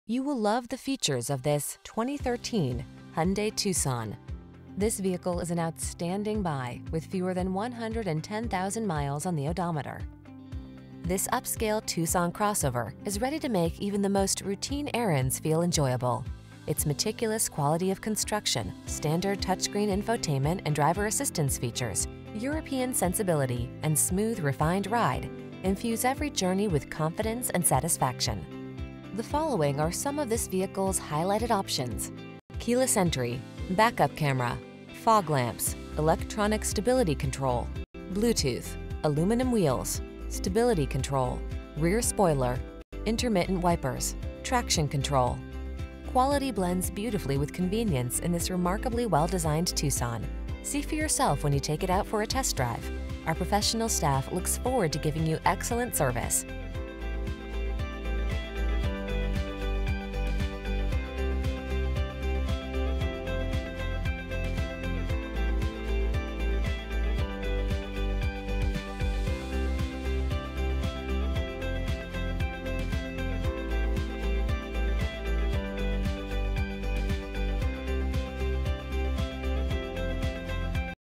2013 Hyundai Tucson El Paso, Las Cruces, Alamogordo, Ruidoso, Carlsbad NH148909B
Graphite Gray Used 2013 Hyundai Tucson available in El Paso, Texas at Hyundai of El Paso. Servicing the Las Cruces, Alamogordo, Ruidoso, Carlsbad, TX area.

2013 Hyundai Tucson GLS Graphite Gray 2.4L I4 MPI DOHC CVVT 16V PZEV Theta 2 6-Speed Automatic with Overdrive AWD Bluetooth, Backup Camera, AWD, 17 x 6.5 Alloy Wheels, 60/40 Split Fold-Down Rear Seatback, Air Conditioning, Bodycolor Rear Spoiler w/LED Brake Lights, Cloth Seat Insert w/Leatherette Bolster, EZ Lane Change Assist, Front fog lights, Heated Front Bucket Seats, Hillstart Assist Control (HAC), Leather Shift Knob, Leather-Wrapped Shift Knob, Leather-Wrapped Steering Wheel, Option Group 1, Radio: Autonet AM/FM/CD/MP3 Audio System, Remote keyless entry, Remote Keyless Entry System w/Alarm, Steering wheel mounted audio controls, Steering-Wheel Mounted Audio & Cruise Controls, Tilt & Telescopic Steering Wheel. Clean CARFAX. 20/27 City/Highway MPGbrbrAwards:br  * 2013 KBB.com 10 Best Used Compact SUVs Under $10,000   * 2013 KBB.com 10 Best Used All-Wheel-Drive Vehicles Under $10,000